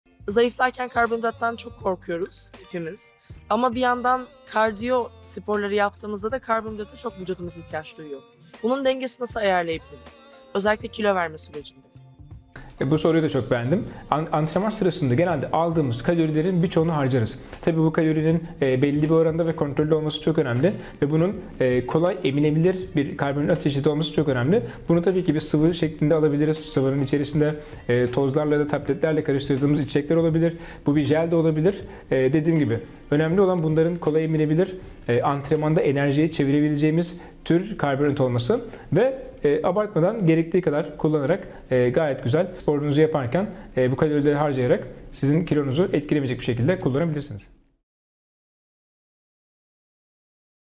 WUPTalks - Orçun Kürüm & Meltem Kılıç / Kardiyo Sporlarında Karbonhidrat Dengesi
Sporcu beslenmesi uzmanı Orçun Kürüm, profesyonel bisikletçi Meltem Kılıç'ın kardiyo sporlarında karbonhidrat dengesinin ayarlanması ile ilgili sorularını cevaplıyor.